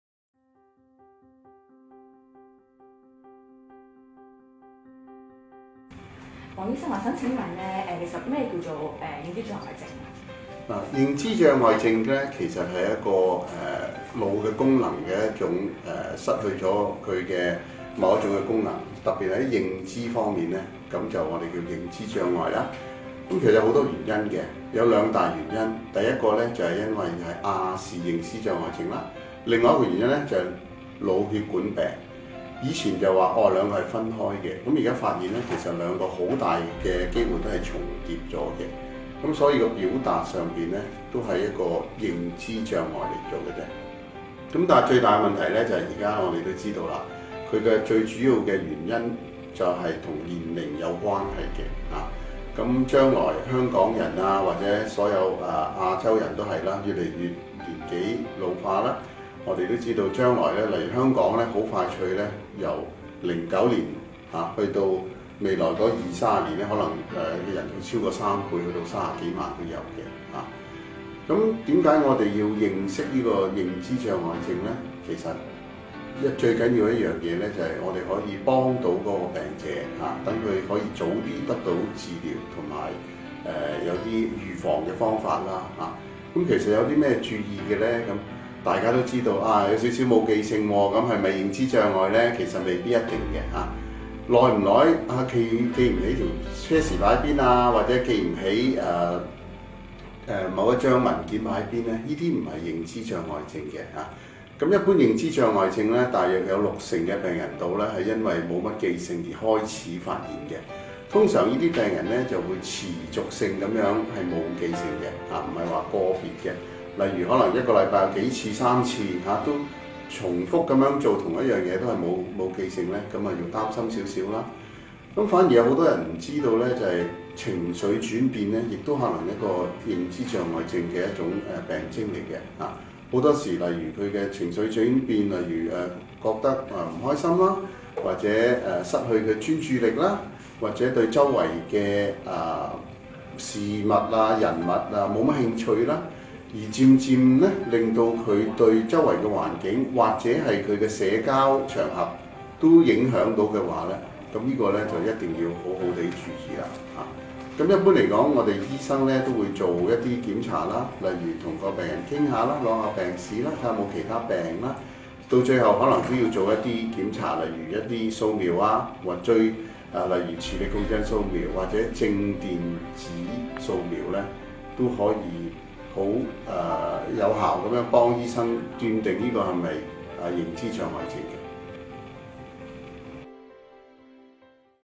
醫學1分鐘：認知障礙症
香港人口老化，認知障礙症患者愈來愈多。據統計，香港每10名70歲以上長者，便有1名患者；85歲以上患病率更高達三分一。認知障礙症，在初發階段是「有跡可尋」，我們應多留意家中長者及早辨悉，避免錯過治療黃金期。以下由腦神經專科醫生黃家星為大家解構「認知障礙症」。